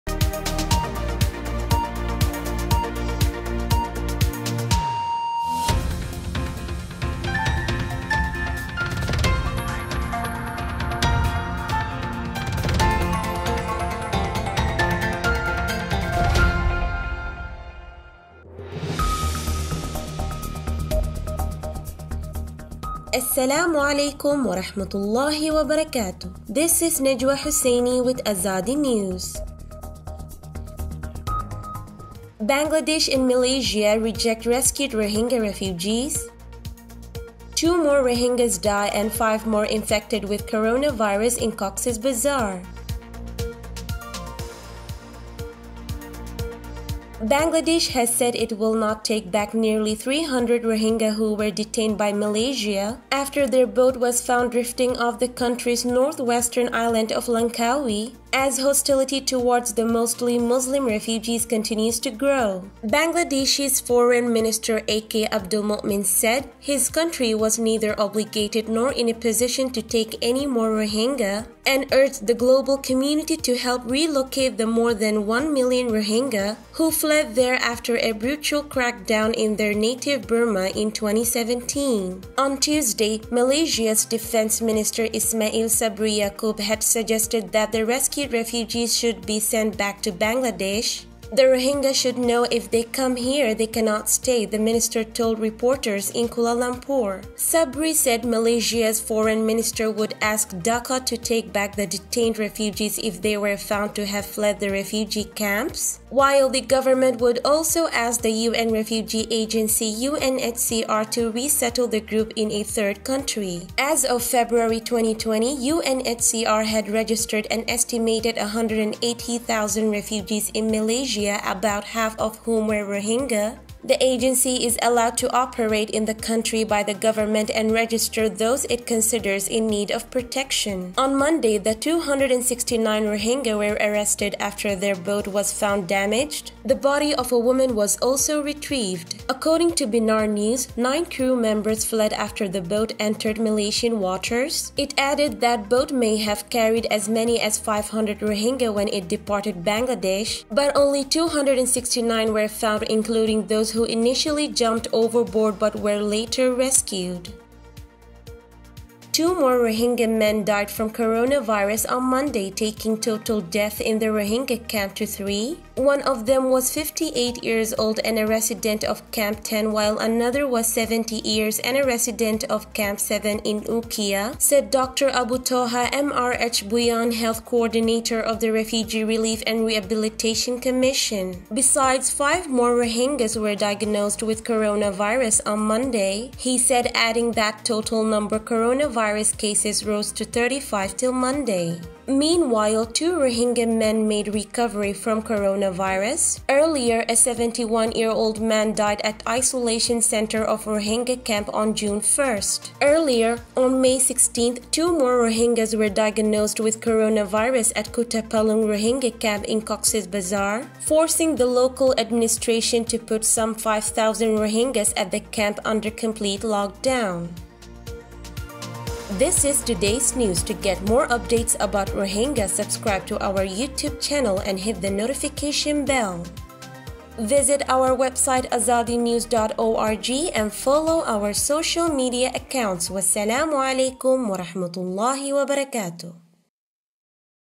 Rohingya English News | 11 June 2020 - Azadi TV
Headlines:
1.
Bangladesh & Malaysia reject rescued Rohingya refugees.
2.
2 more Rohingyas die & 5 more infected with Coronavirus in Cox's Bazar.